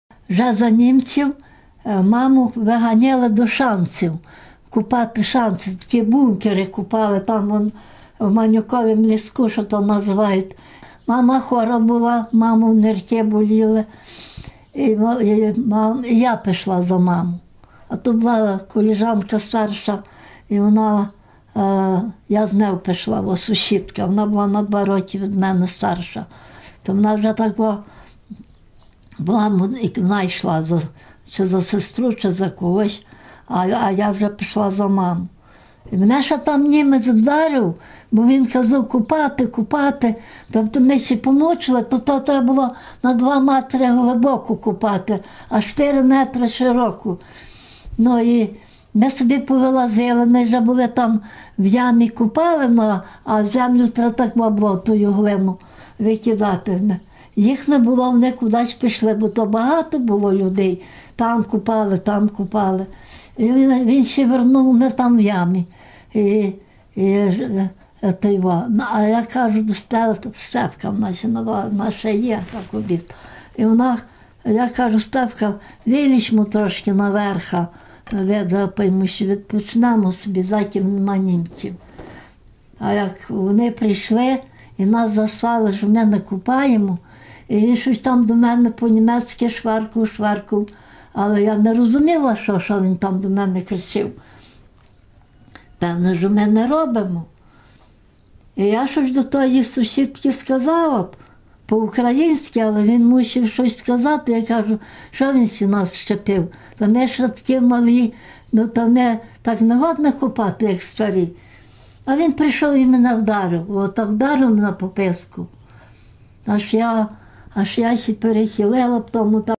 Марія Кушнір про примусові роботи у період німецької окупації
Оповідач: Марія Кушнір, 1930 р. н.
с. Кимир Перемишлянського р-ну Львівської обл.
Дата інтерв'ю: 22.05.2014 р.
Інтерв'юери: Марія Мухар, Віталія Пономарьова